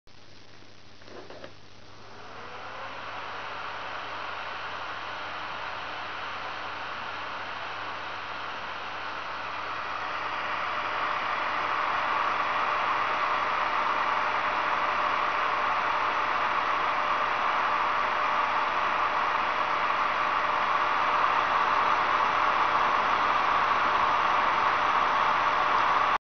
Lüftung BR 242 aus Neu-Hennigsdorf
Die Lüftungsgeräusche sind ECHT, keine Soundkarte!